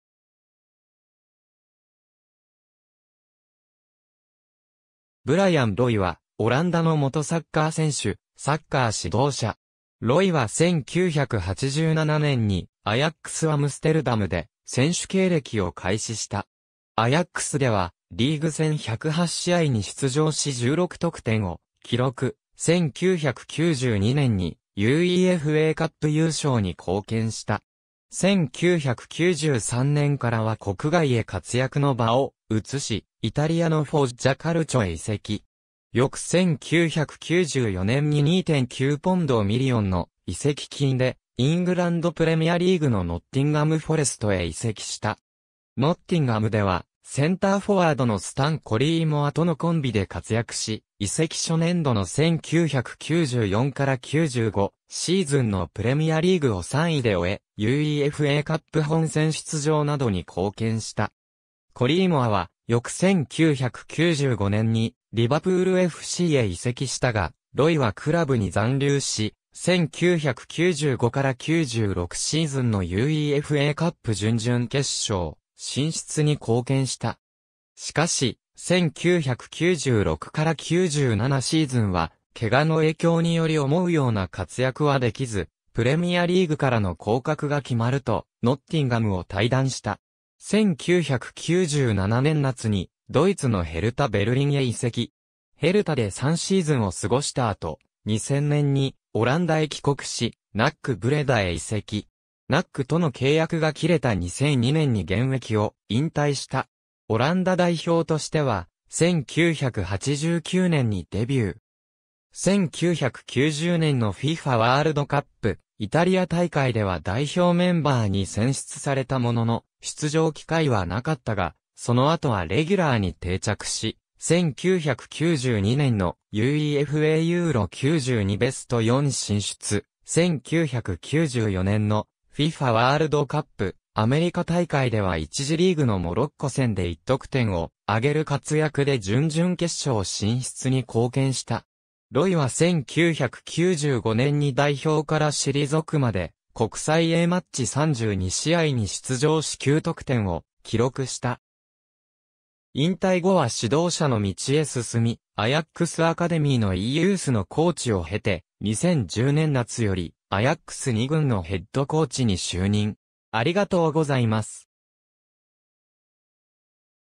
ブライアン・ロイ
アヤックス・アムステルダムの選手
カルチョ・フォッジャ1920の選手
ヘルタ・ベルリンの選手
ブライアン・ロイ（Bryan Roy、1970年2月12日 -）は、オランダの元サッカー選手、サッカー指導者。
ロイは1987年にアヤックス・アムステルダムで選手経歴を開始した。
アヤックスではリーグ戦108試合に出場し16得点を記録、1992年にUEFAカップ優勝に貢献した。
1993年からは国外へ活躍の場を移し、イタリアのフォッジャ・カルチョへ移籍。
翌1994年に￡2.9millionの移籍金でイングランド・プレミアリーグのノッティンガム・フォレストへ移籍した。
ノッティンガムではセンターフォワードのスタン・コリーモアとのコンビで活躍し、移籍初年度の1994-95シーズンのプレミアリーグを3位で終え、UEFAカップ本選出場などに貢献した。
コリーモアは翌1995年にリヴァプールFCへ移籍したが、ロイはクラブに残留し、1995-96シーズンのUEFAカップ準々決勝進出に貢献した。
しかし、1996-97シーズンは怪我の影響により思うような活躍は出来ず、プレミアリーグからの降格が決まると、ノッティンガムを退団した。
1997年夏にドイツのヘルタ・ベルリンへ移籍。
ヘルタで3シーズンを過ごした後、2000年にオランダへ帰国し、NACブレダへ移籍。
NACとの契約が切れた2002年に現役を引退した。
オランダ代表としては、1989年にデビュー。
1990年のFIFAワールドカップ・イタリア大会では代表メンバーに選出されたものの出場機会はなかったが、その後はレギュラーに定着し、1992年のUEFA EURO '92ベスト4進出、1994年のFIFAワールドカップ・アメリカ大会では1次リーグのモロッコ戦で1得点を挙げる活躍で準々決勝進出に貢献した。
ロイは1995年に代表から退くまで国際Aマッチ32試合に出場し9得点を記録した。
引退後は指導者の道へ進み、アヤックス・アカデミーのEユースのコーチを経て、2010年夏よりアヤックス２軍 (いわゆるヨング・アヤックス) のヘッドコーチに就任。